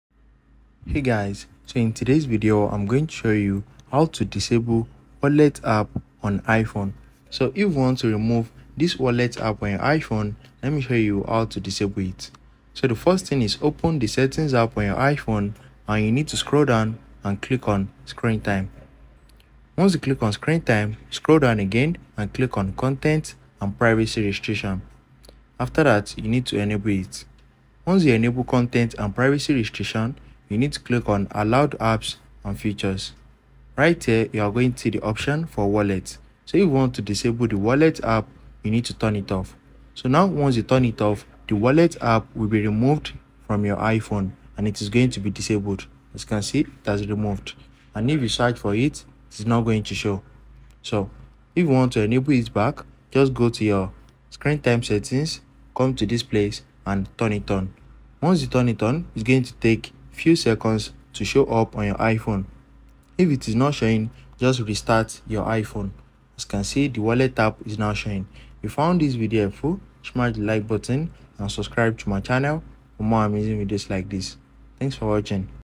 How to Disable Wallet on iPhone
Learn how to disable the Wallet app on your iPhone with this straightforward tutorial! If you want to remove or deactivate the Wallet app for privacy, security, or personal preference, this video will guide you through the necessary steps. Perfect for iPhone users who want to manage their apps and settings effectively. Follow our easy instructions to disable the Wallet app on your iPhone today.